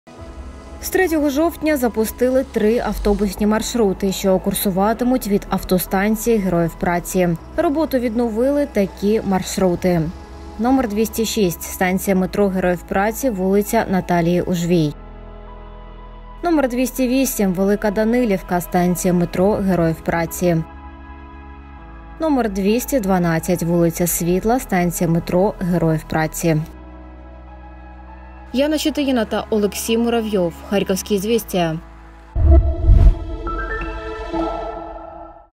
На Салтівці відновили роботу три автобусні маршрути
Три автобусні маршрути запрацювали сьогодні, 3 жовтня. Автобуси будуть ходити з автостанції «Героїв Праці» у Харкові.

Як повідомили у Департаменті інфраструктури, роботу відновили такі маршрути:

№06е «ст. м. «Героїв Праці» – вул. Наталії Ужвій»,
№208е «Велика Данилівка – ст. м. «Героїв Праці»,
№ 212е «Вул. Світла – ст. м. «Героїв Праці».